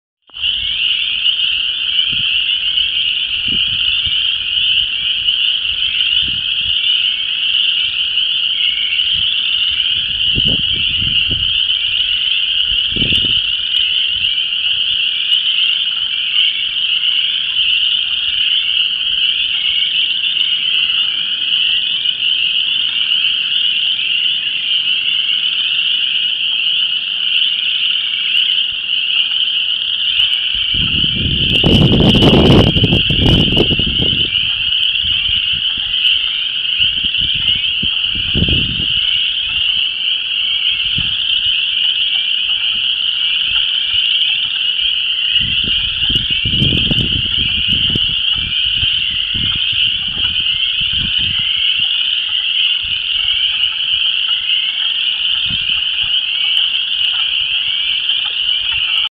Vermilion County Frog Chorus
Frog love music of a decent sized disturbed ephemeral depression within Vermilion Co. IL.

Chorus Frogs, Spring Peepers, and Wood Frogs can be heard through the wind noise.